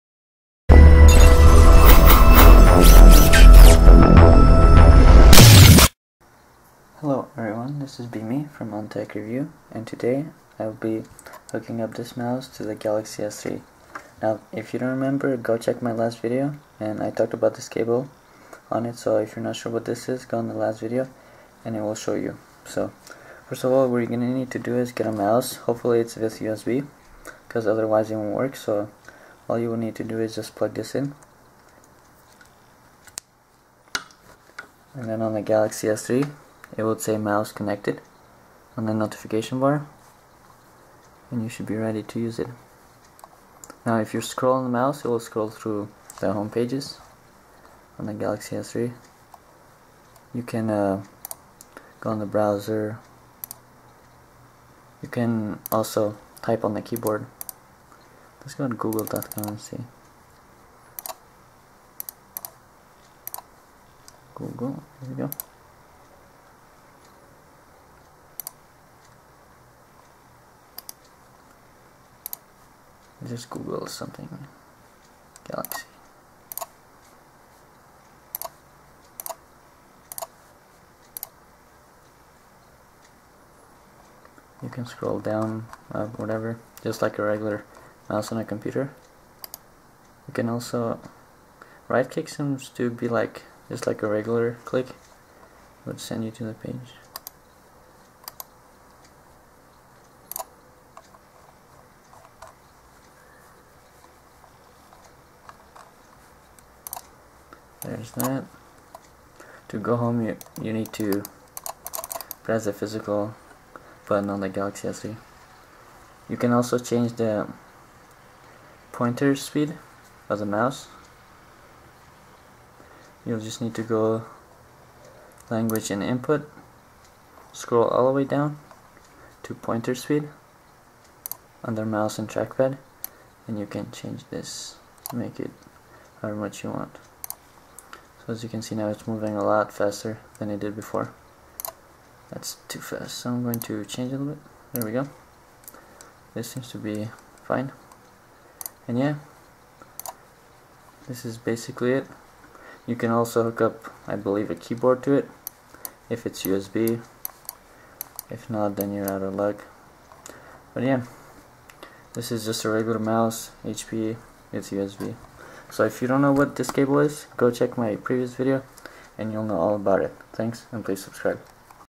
Galaxy S3 - USB Host - Connecting a mouse - Quick tip
Galaxy S3 Accessories - USB Host - Connecting a mouse.
This is just a quick tip on how to connect a mouse to the galaxy s3. If you are not sure what the cable is, please check my previous video. If you guys have any questions, drop it on the comments below. If you guys want to see anything else, drop it on the comments below. I'll try to make it happen. Thanks.